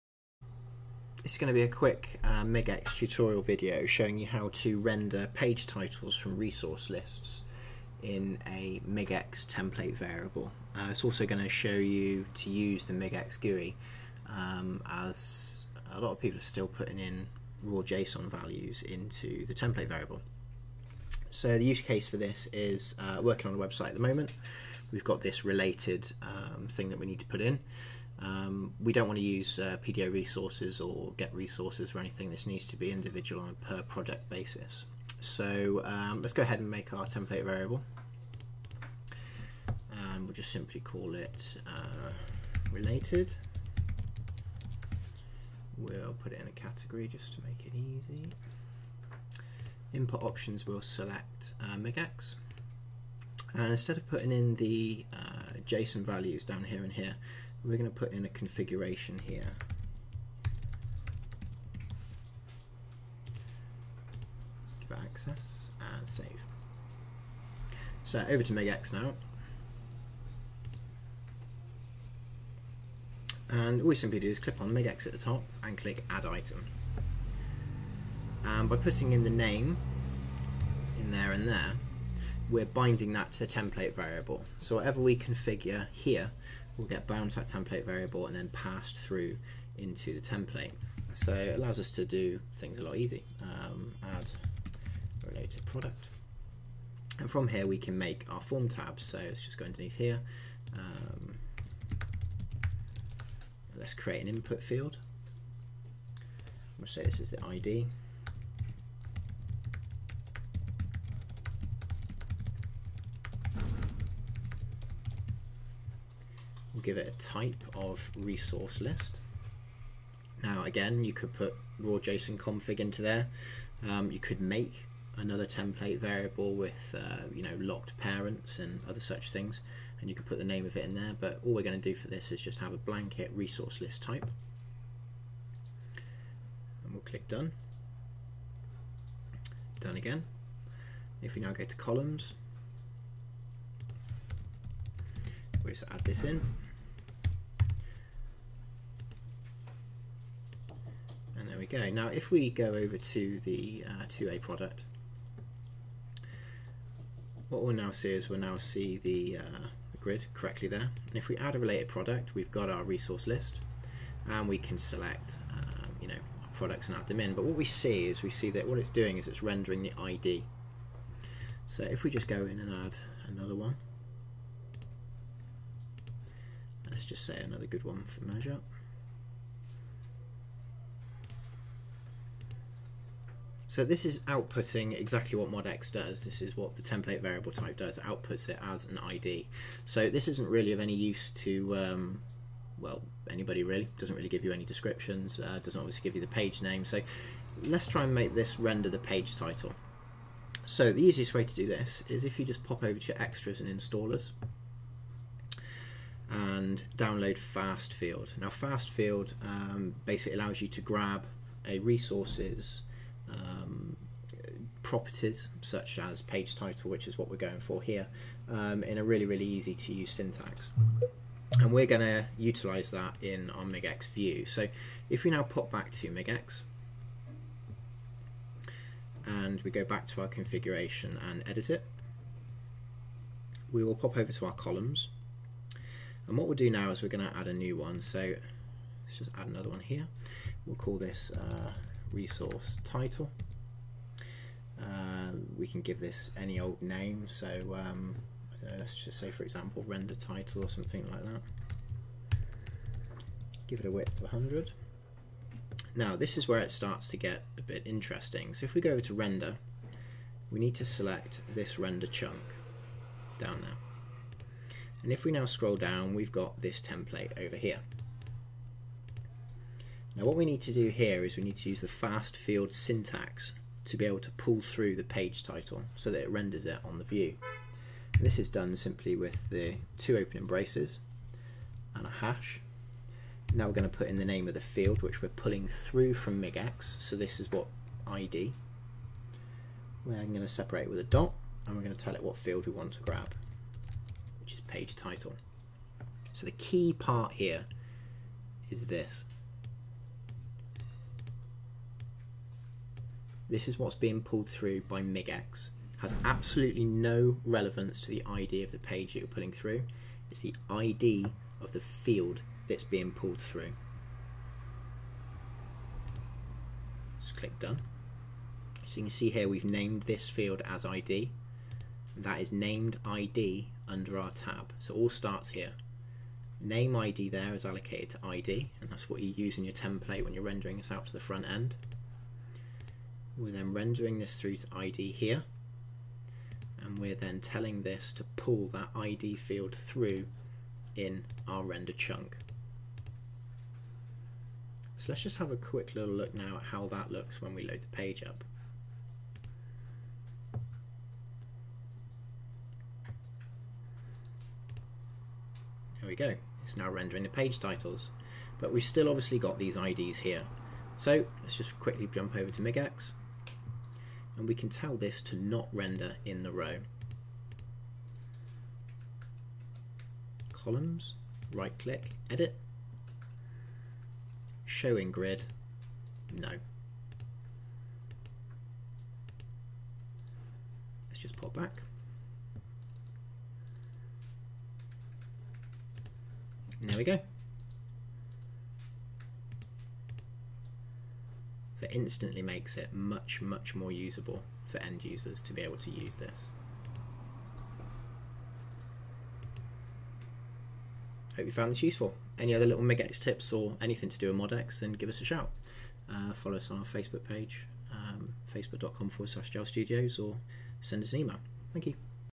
MODX MIGX Resource List ID's as Pagetitle
This video shows you how to get a MIGX field resource list ID to display the page title of the selected document. It also gives  you a brief overview of using the MIGX GUI to build your migx template variables.